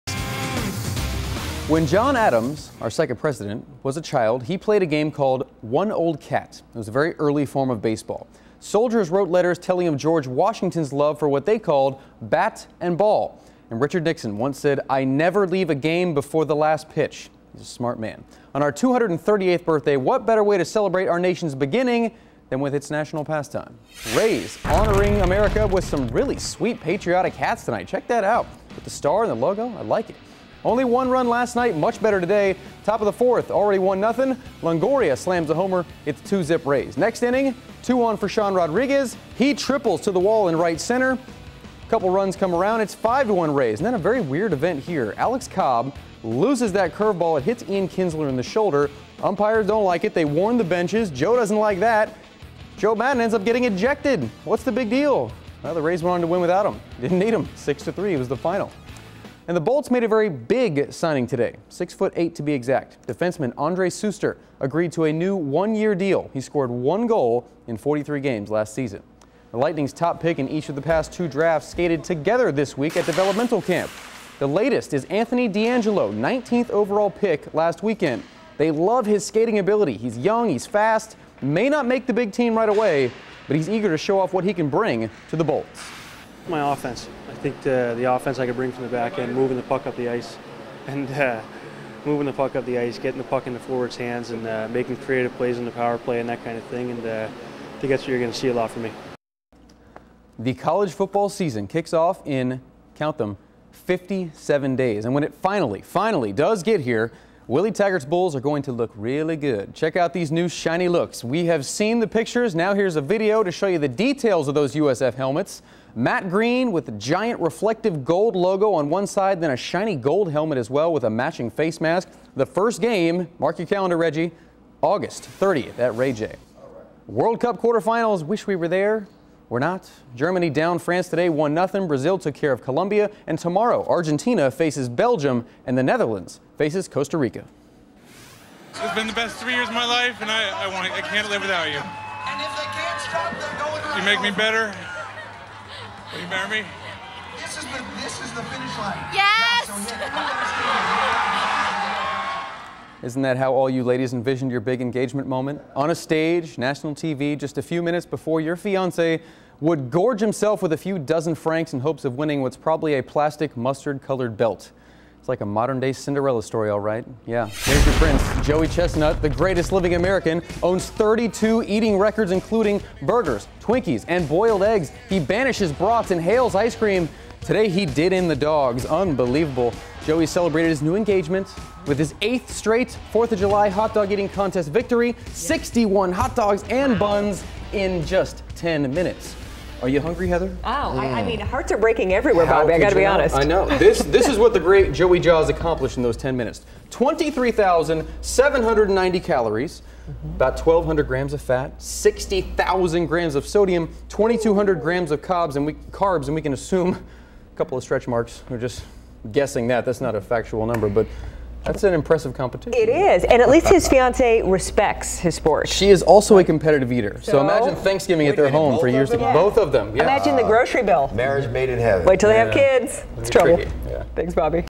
Bobby Lewis 4th of July sportscast
A proposal, Hot Dogs, Bolts and SWEET new USF helmets